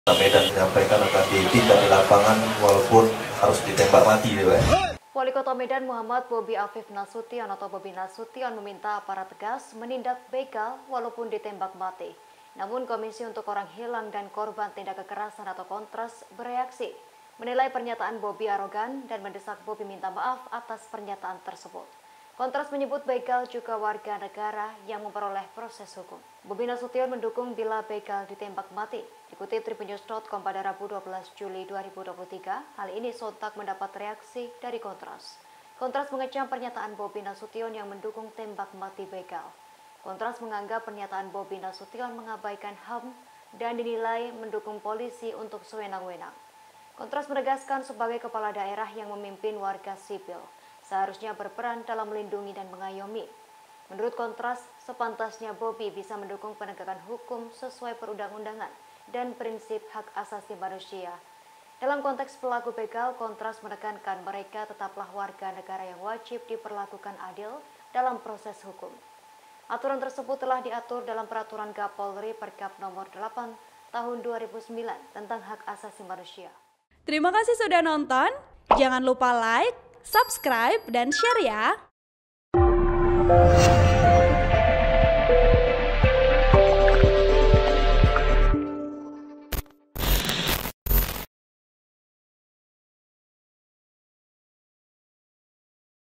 Bobby Nasution Minta Polisi Langsung Dor Begal hingga Tewas, KontraS: Begal Juga Warga Negara
TRIBUN-VIDEO.COM - Wali Kota Medan, Muhammad Bobby Afif Nasution meminta aparat tegas menindak begal walaupun ditembak mati.

Namun Komisi untuk Orang Hilang dan Korban Tindak Kekerasan (KontraS) bereaksi, menilai pernyataan Bobby arogan dan mendesak Bobby minta maaf atas pernyataan tersebut.

KontraS menyebut, begal juga warga negara yang memperoleh proses hukum.

Bobby Nasution mendukung bila begal ditembak mati.

Dikutip Tribunnews.com, Rabu (12/7/2023), hal ini sontak mendapat reaksi dari KontraS.

KontraS mengecam pernyataan Bobby Nasution yang mendukung tembak mati begal.

KontraS menganggap pernyataan Bobby mengabaikan HAM dan dinilai mendukung polisi untuk sewenang-wenang.

KontraS menegaskan, sebagai kepala daerah yang memimpin warga sipil seharusnya berperan dalam melindungi dan mengayomi.

Menurut KontraS, sepantasnya Bobby bisa mendukung penegakan hukum sesuai perundang-undangan dan prinsip HAM.

Dalam konteks pelaku begal, Kontras menekankan mereka tetaplah warga negara yang wajib diperlakukan adil dalam proses hukum.

Aturan tersebut telah diatur dalam Peraturan Kapolri (Perkap) Nomor 8 Tahun 2009 tentang HAM.

(Tribun-Video.com/ TribunJabar.id)

Artikel ini telah tayang di TribunJabar.id dengan judul Kontras Bereaksi dan Kecam Pernyataan Wali Kota Medan, Dianggap Bakal Langgar HAM Soal Begal,

VP: Dimas